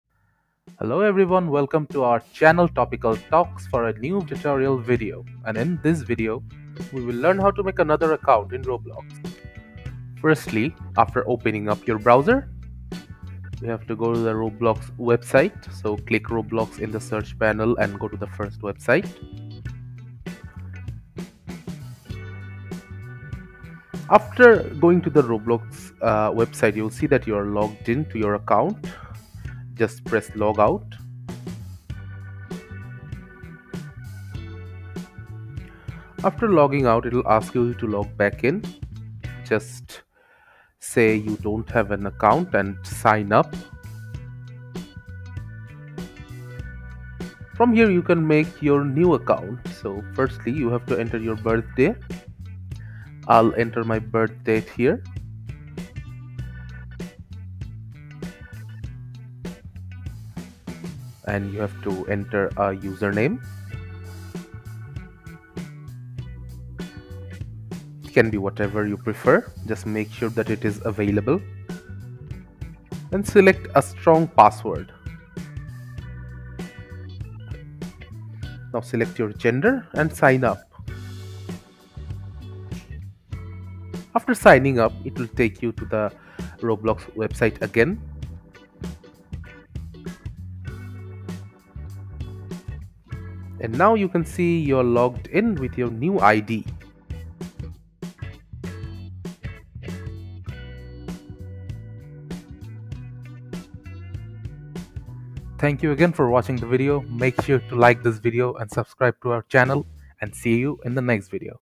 How To Get Multiple Accounts In Roblox (2025 UPDATED)
If you want to use multiple accounts under one account, then learn how you can create another account and switch roblox account easily on a PC or laptop.